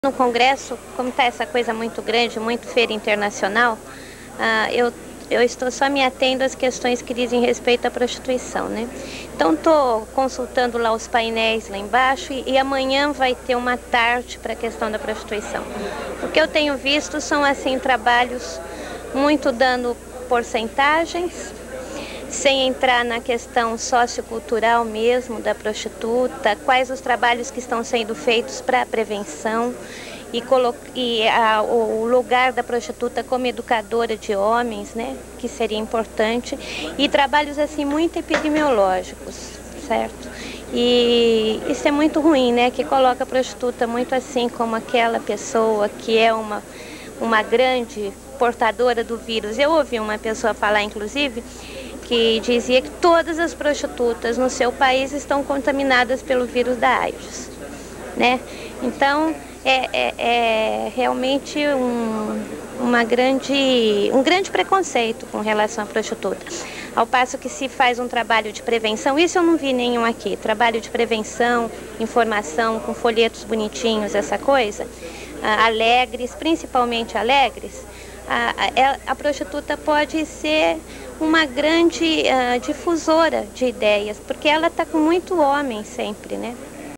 Depoimento de Gabriela Leite no Congresso Internacional de AIDS em 1989, Montreal
Congresso International de AIDS 1989 em Montreal. Video encontrado no arquivo do Centro de Documentação (CEDOC) da Associação Brasileira de AIDS (ABIA).